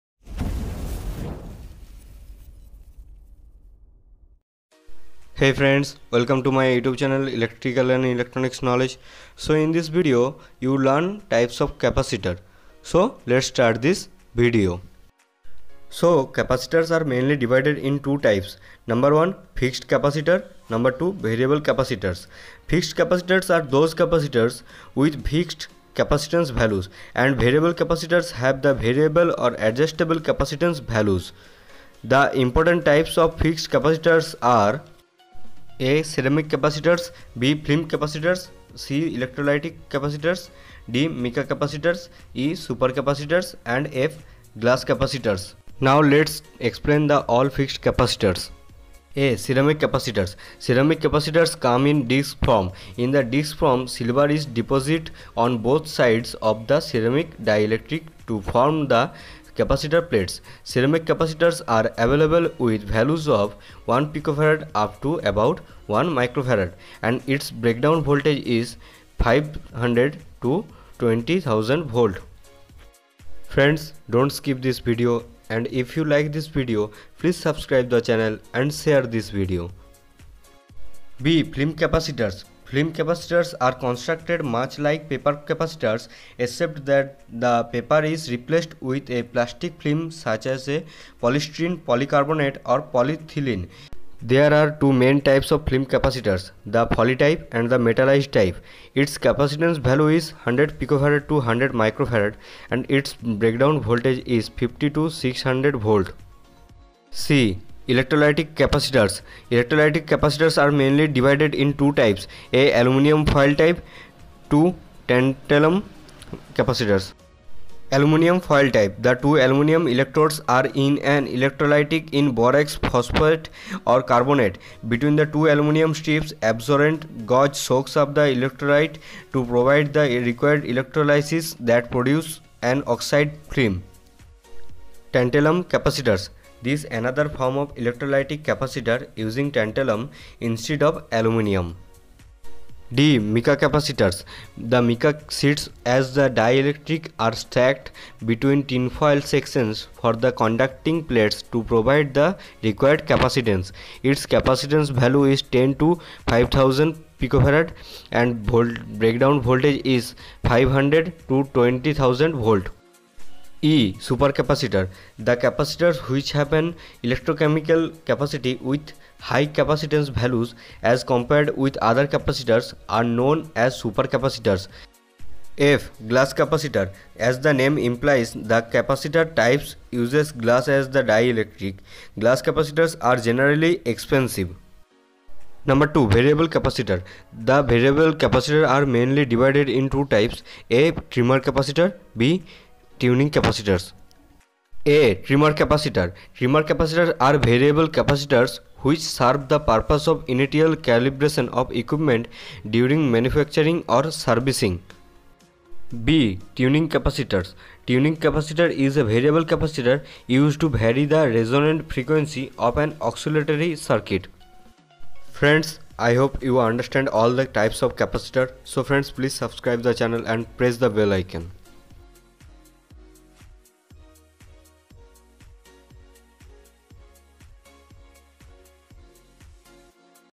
Types of Capacitor | Full Explanation in English
In this channel you can learn all about electrical and electronics technology and how to make projects.

Different Types of Capacitors :
There are various types of capacitor that are used in electrical and electronic circuits but here is the list of widely used capacitor types.
1. Ceramic capacitors
2. Film capacitors
3. Electrolytic capacitors
4. Mica capacitors
5. Super Capacitors
6. Trimmer Capacitors ( variable capacitor)
7. Tuning capacitors ( variable capacitor)
Other useful videos are -

Hello friends,
About this video- In this video ,I can explain different types of Capacitors.full explanation in English .

this video will be helpful to all students of science and Engineering in understanding the various types of Capacitors with explanation in English.